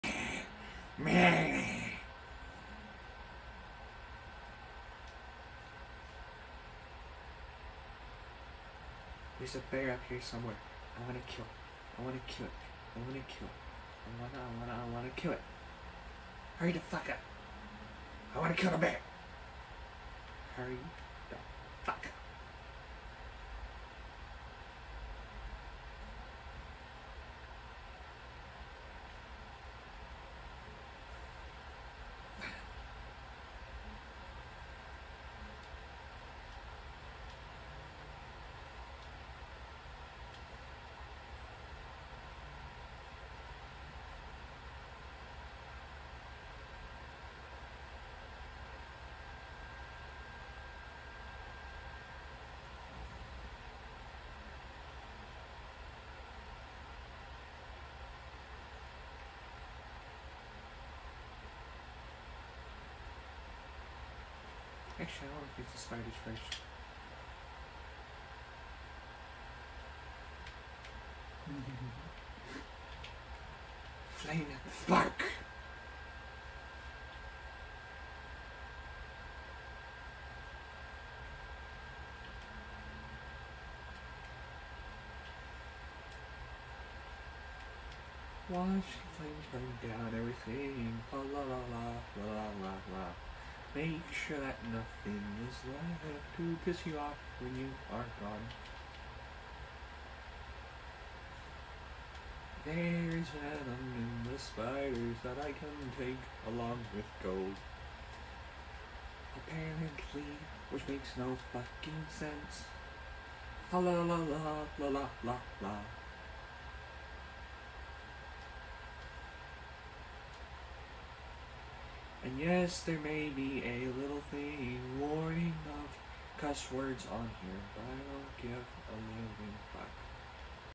Surviving the Wild: Bear Encounter and Chaos
Join me in an unpredictable adventure as I navigate the wilderness, facing the daunting presence of a bear! This wild journey is filled with unexpected moments and vivid imagery – witness the struggle of man versus nature. Don't miss the fiery chaos!